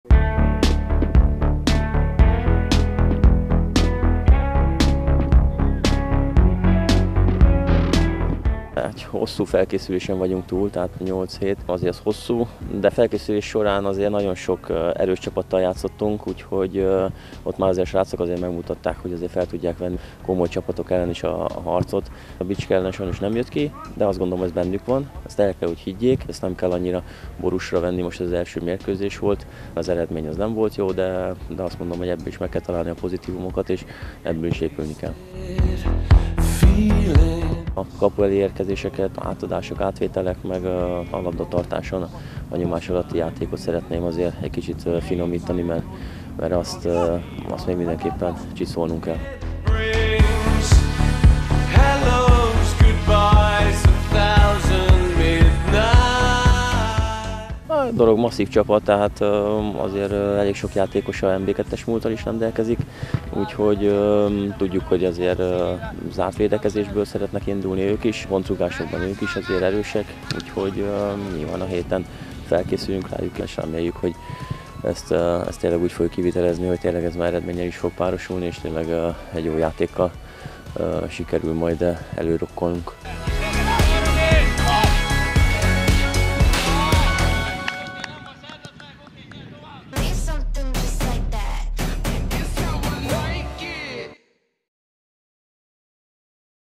A Dorog lesz az első idei vendég az NB III-ban
Vanczák Vilmos edző nem borús a bicskei vereség miatt, tudja, hogy jól dolgoztak a srácok, de pár területen előrelépést vár az első meccshez képest.